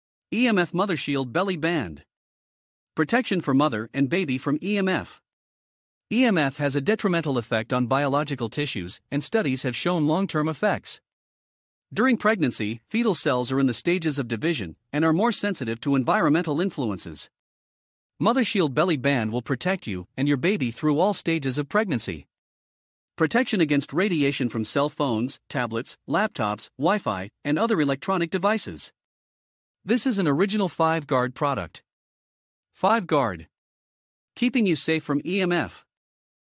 EMF Radiation Protection Maternity Belly Band
Pregnancy EMF Radiation Protection Baby Belly Band.
Supportive and super flexible and incredibly soft.
Designed to accompany you safely through all stages of pregnancy.
Outer layer is made of premium 100% spandex fabric.
Inner protective lining is made of pure 100% silver fiber fabric.

Cellular radiation has a detrimental effect on biological tissues. Mother-shield Belly Band protects from cell phones, tablets, laptops, WiFi and other electronic devices radiation.

The product comes in two sizes, and is stretched to fit perfectly.

Give your baby perfect radiation protection.

This product is made of Silver Fiber that is conductive and can block radiation and also has antibacterial characteristics. Silver Fiber technology has an Amazing shielding performance 10MHz to 4GHz.

EMF protection from 4G-5G cell towers, Wi-Fi, Bluetooth, microwaves, cordless phones, electrical wires, Household Appliances, and more.

Why you must have 5G Protection
5G technology requires more antennas to be placed anywhere in the public space, and its radiation power is up to ten times stronger than that of previous generations! Beware of imitations! This is an original 5 GUARD product.

Shipment: worldwide.